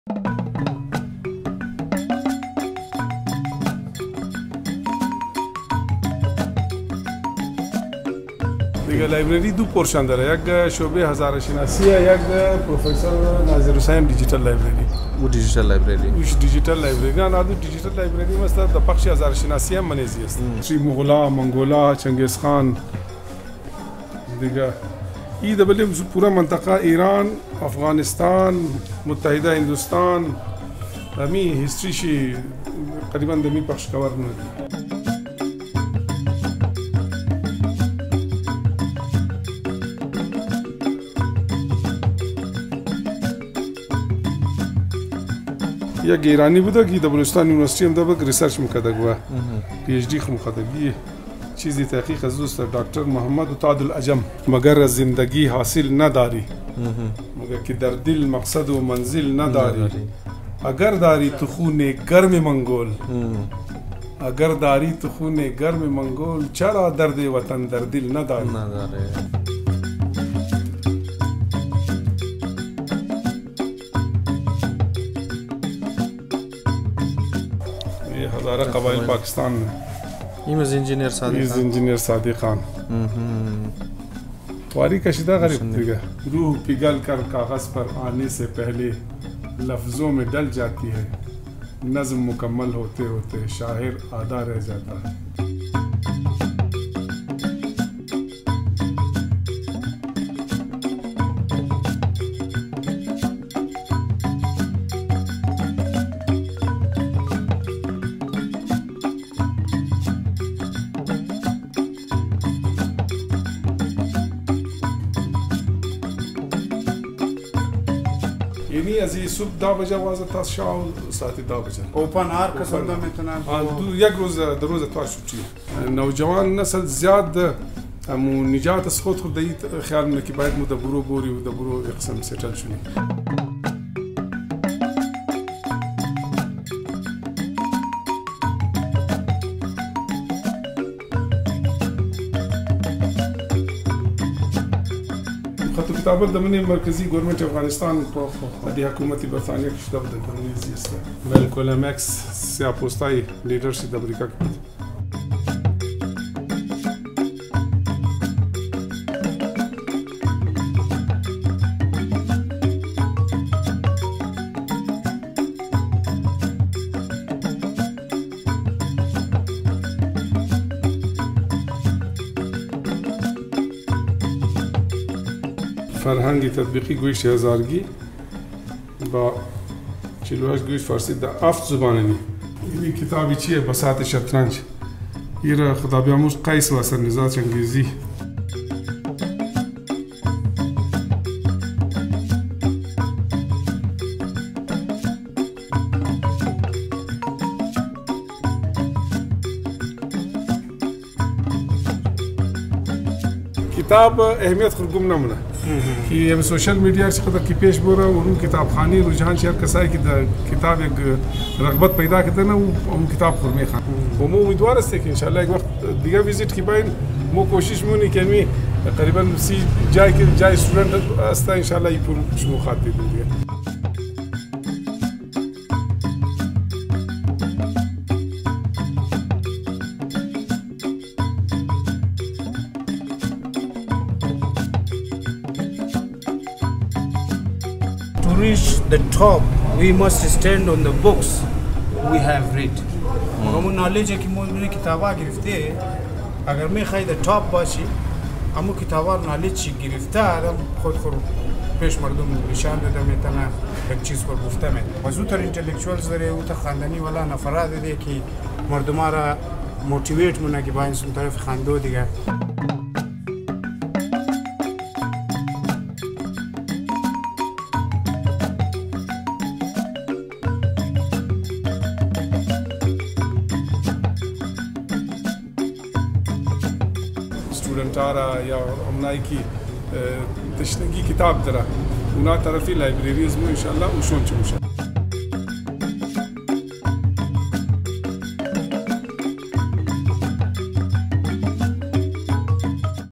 DIGITAL LIBRARY | پروفیسرناظرحسین
Professor | NAZIR HUSSAIN | Tamgha-e-Fazilat Pakistan DIGITAL LIBRARY | CONNECT TO YOUR INNERSELF

Host | Syed Naimatullah
Guest | Nadir Hazara
Venue | Tanzeem Nasl e Nau Hazara Mughul Quetta
Cameraman & Editor | M.H.Chagar